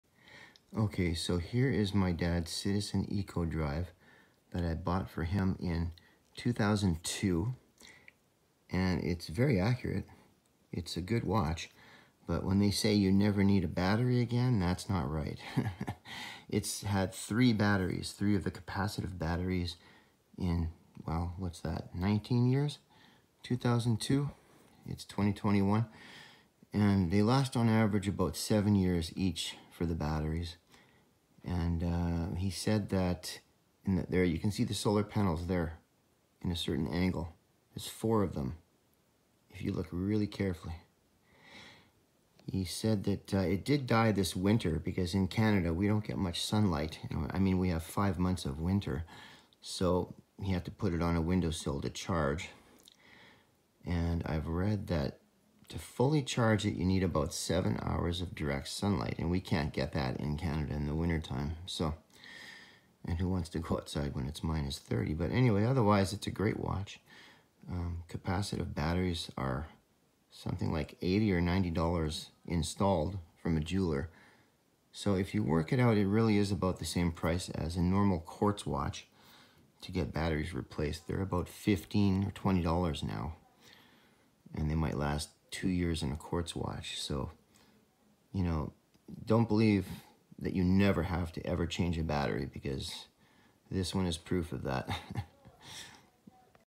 2002 Citizen Eco Drive Watch
2002 Citizen Eco Drive required three capacitive batteries in 19 years so yes you will have to change the batteries eventually despite what Citizen says.